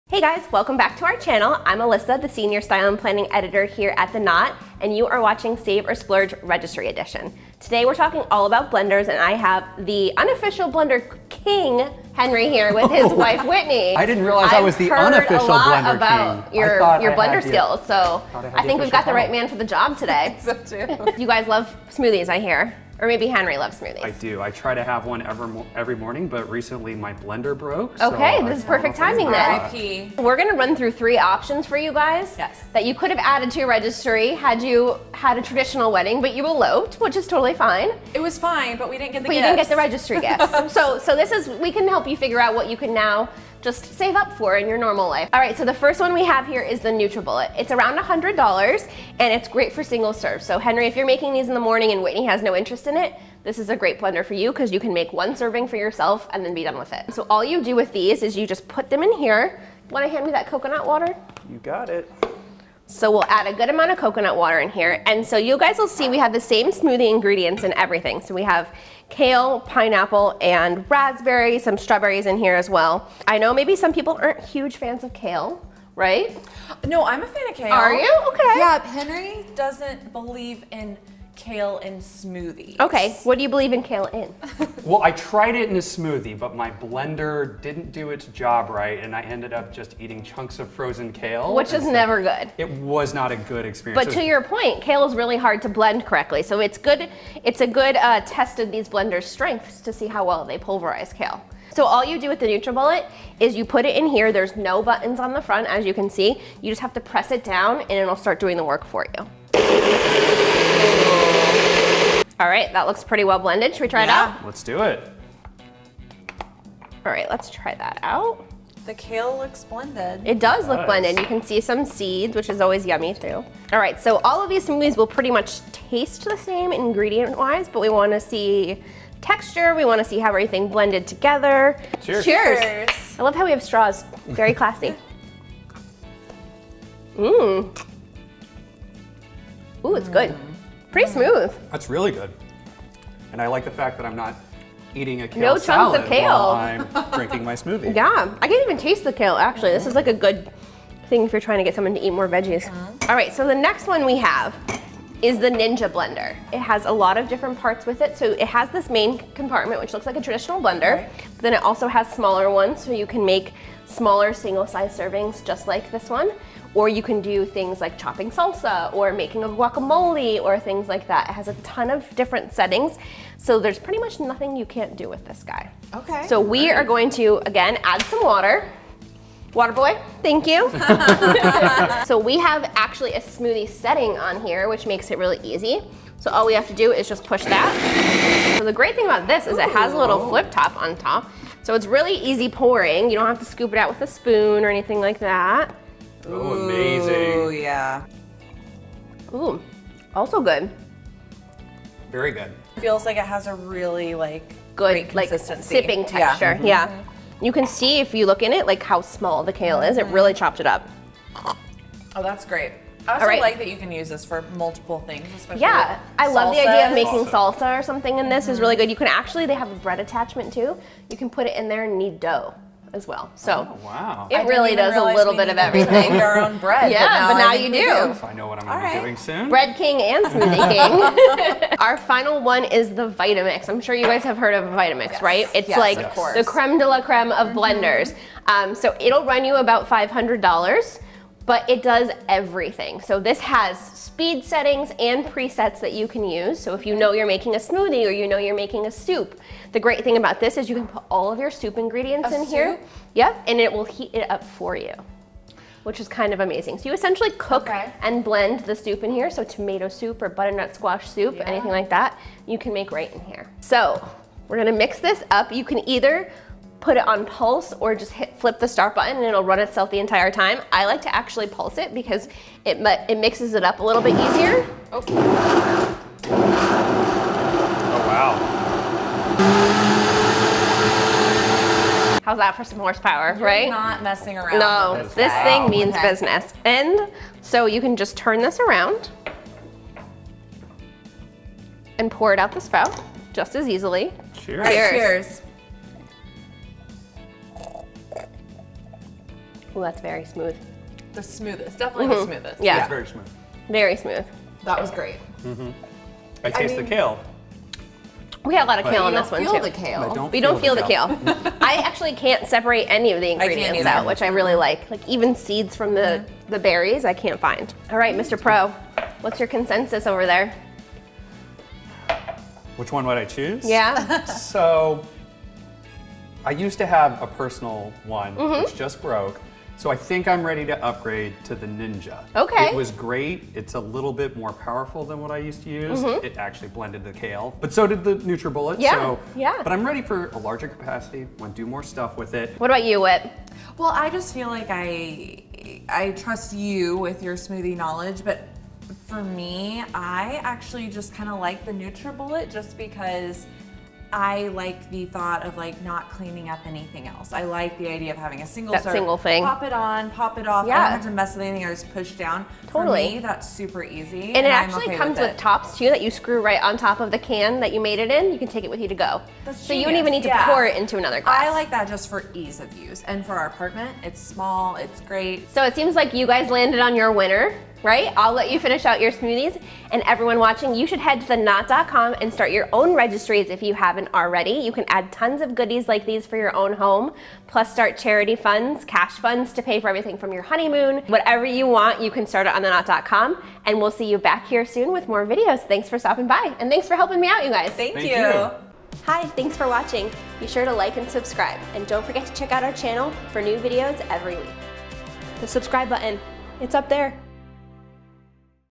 Save or Splurge: Episode 5 - Blenders
A smoothie makes a well balanced meal for any time of the day. However, most blenders on the market don't just have the ability to make smoothies but so much more, making it the perfect item to add to your wedding registry. So in our final episode in our save or Splurge: Registry Edition series, we are testing out three blenders from popular brands like NutriBullet, Ninja, and Vitamix, all ranging in different prices. Our registry experts will discuss the unique features of each blender and then pick which item is their favorite and which item they will be adding to their wedding registry. Keep on watching to see whether the inexpensive or expensive option is the best when it comes to blenders. Don't forget to  check out more episodes in our save or splurge series below.